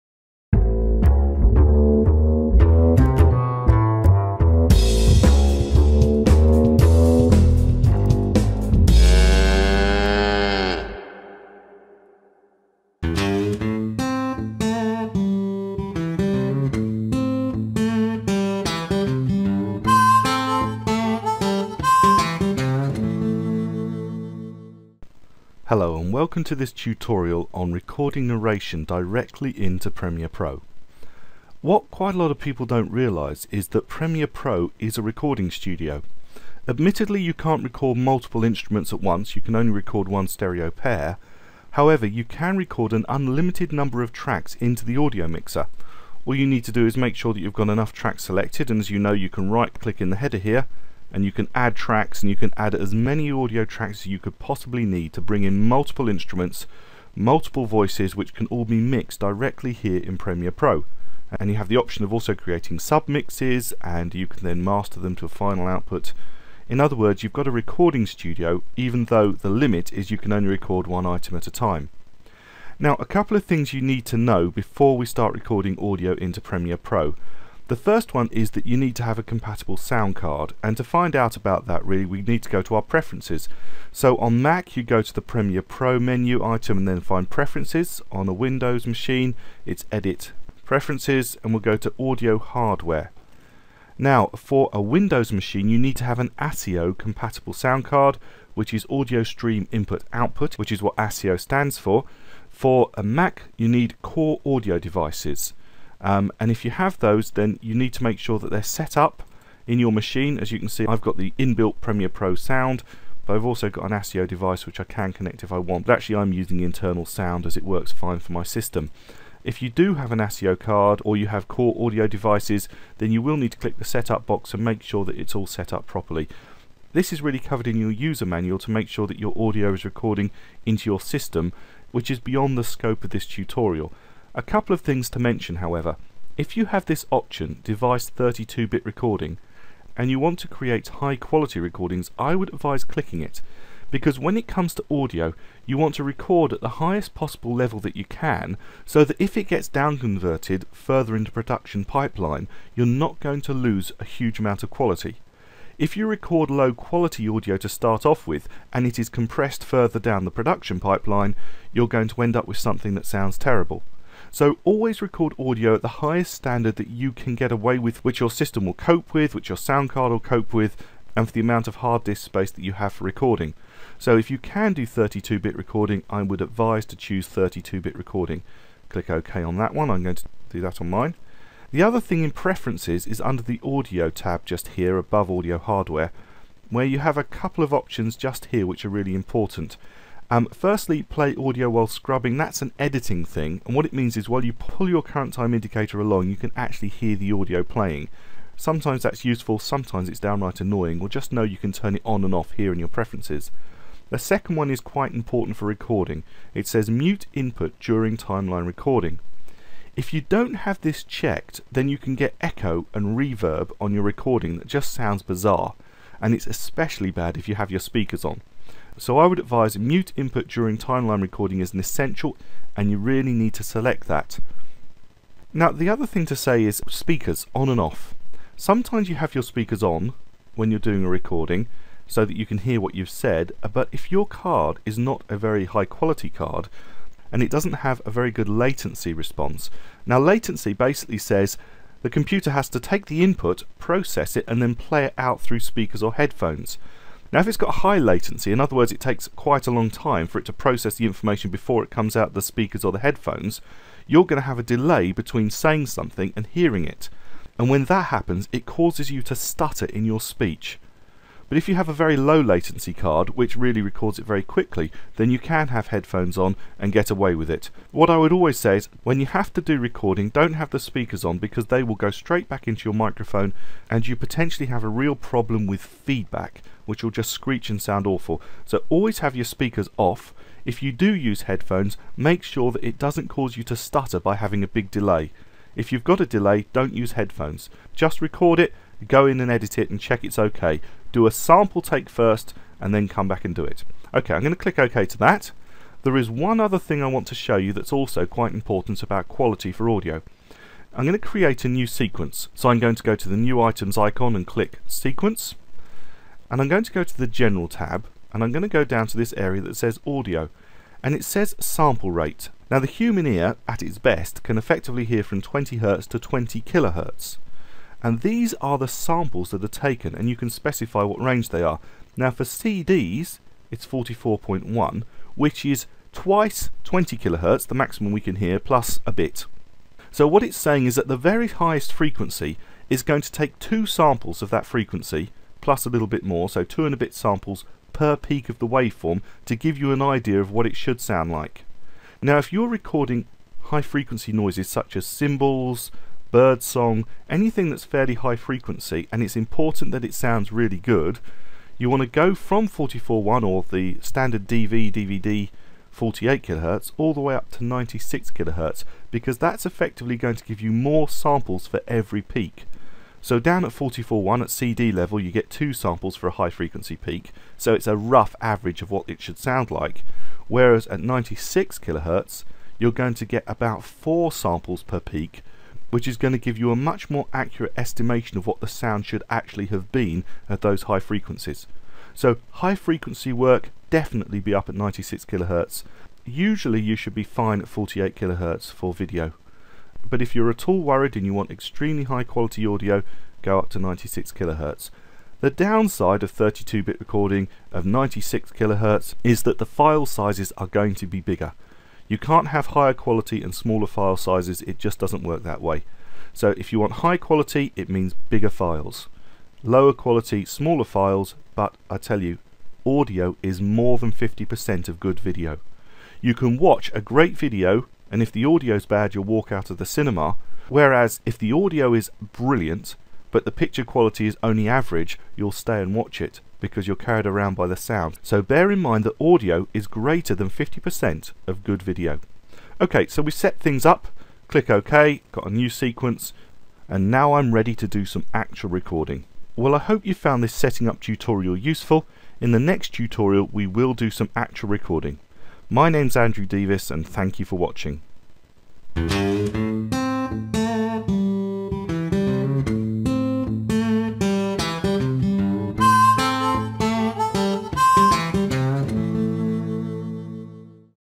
Audio Recording 1: Setting Up to Record Narration
In this PP video tutorial, Andrew Devis shows how to set up Premiere Pro for audio recording, while looking at the options and explaining why they are important.